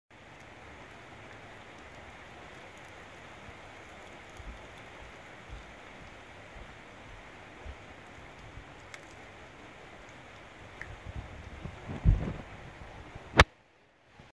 Max munching mouse
Max devours his prey